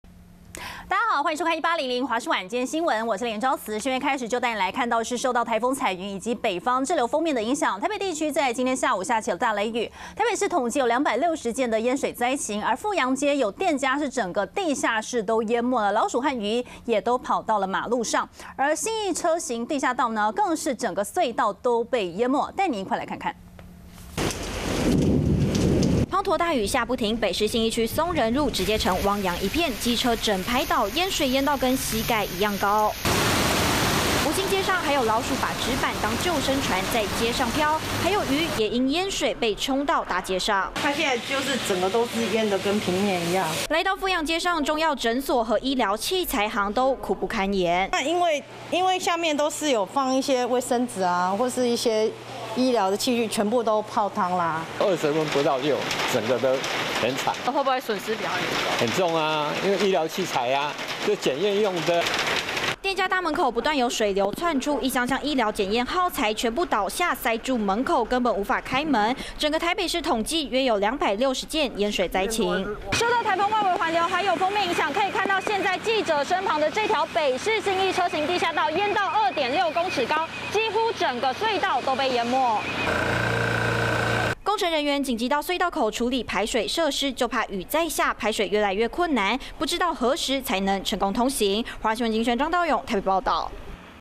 雨彈來襲! 北部下大雷雨多處淹大水｜華視新聞 20210604
馬上帶您來關心，受到颱風彩雲以及北方滯留鋒面影響，台北地區今(4)日下午下起大雷雨，台北市統計有260件淹水災情，富陽街有店家整個地下室都淹沒，老鼠和魚也跑到馬路上，而信義車行地下道更是整個隧道都被淹沒。